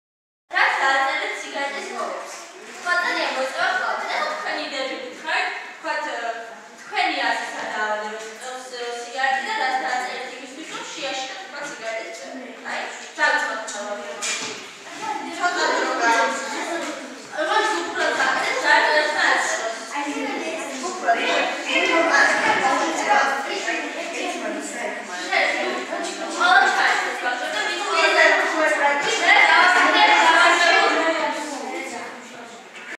ლიდერთა კლუბი "ევრიკა"
დაწყებითი საფეხურის მოსწავლეებთან მუშაობა
სიგარეტი მავნებელია